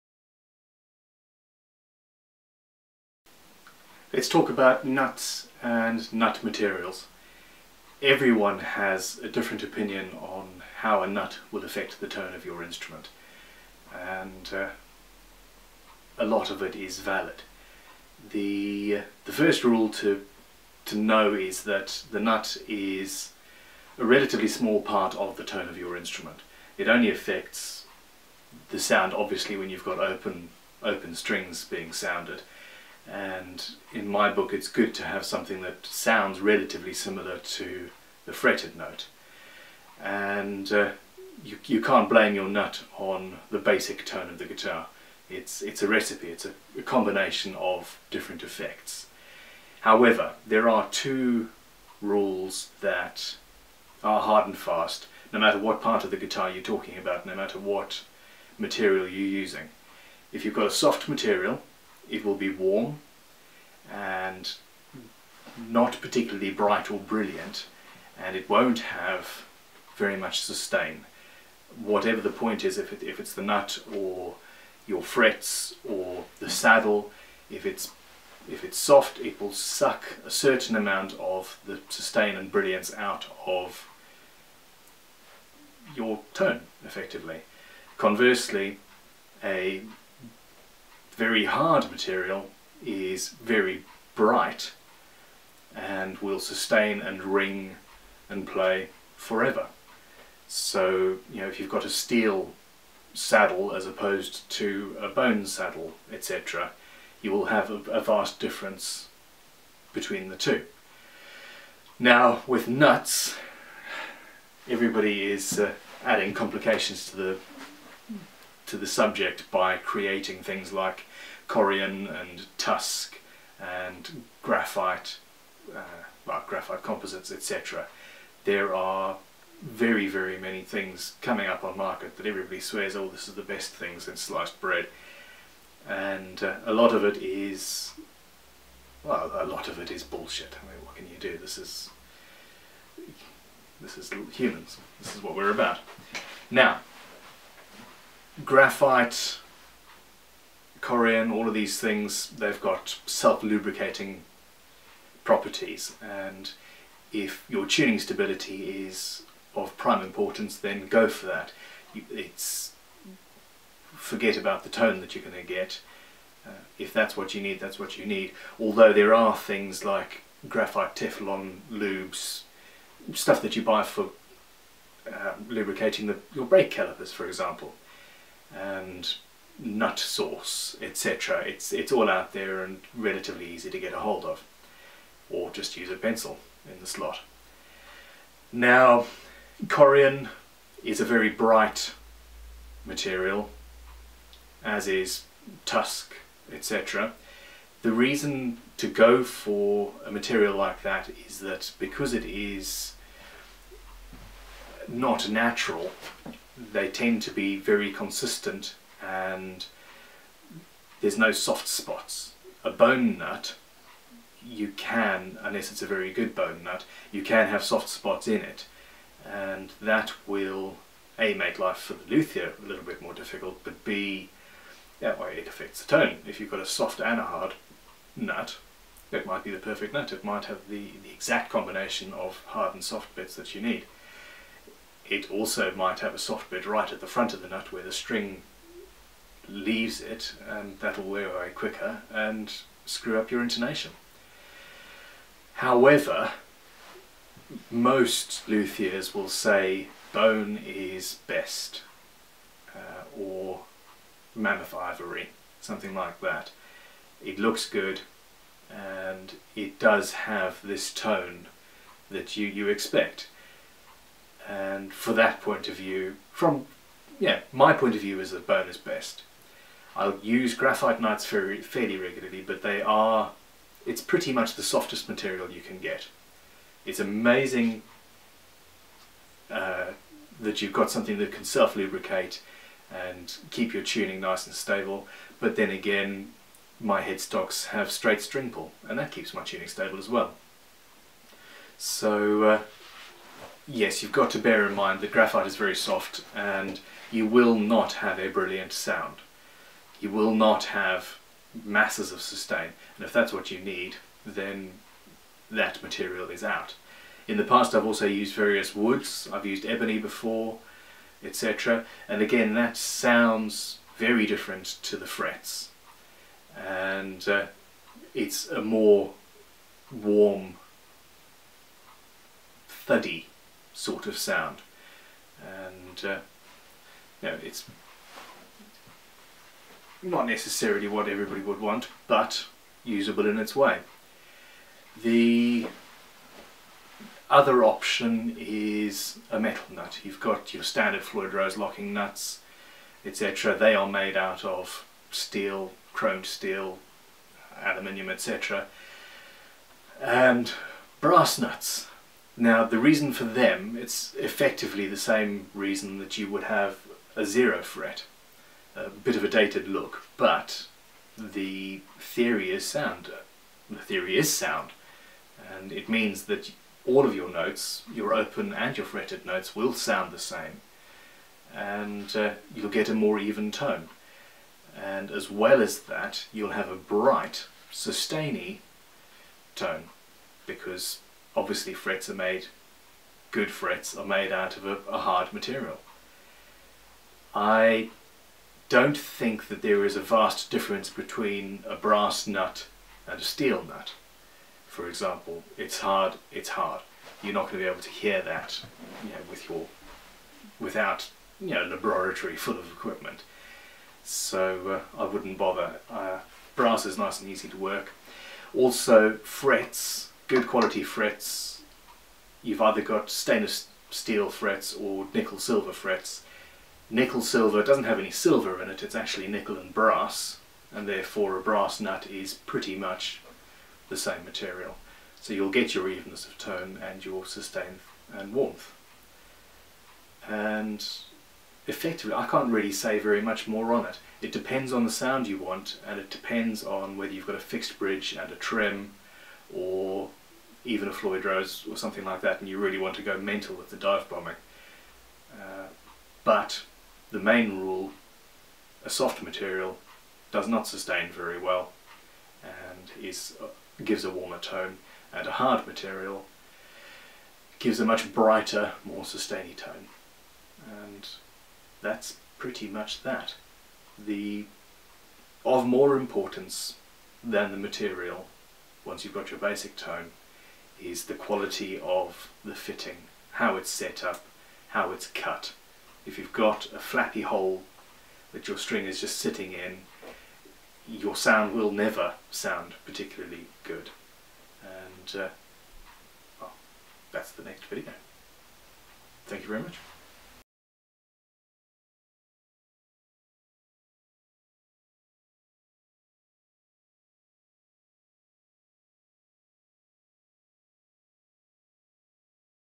Tricks and Tutorials - Guitar Nut Materials and Their Tonal Properties
In this episode, Ben discusses various modern and traditional guitar nut materials and how each material affects the tone of the guitar on which it is fitted. Nothing to do with tone is an exact science but Ben outlines a few general rules to apply when considering the material for your nut.

            Chapters
0:00    Introduction
0:05    The part different materials play in the tone - relatively small
1:55    Discussion of the different materials
7:33    Soft material gives a warmer tone but does not sustain well
            Hard material gives a brighter more sustained tone
7:46    Much  more important is the quality of the fitting, cut, set up
8:11    Conclusion
We'd like to thank our partners for helping to make this video series possible.

Get the Freshest CA glues in the market (USA orders)

As always, thank you so much to our Patrons, who help make videos like this possible.

Thank you again for all your support, we really appreciate it! – Ben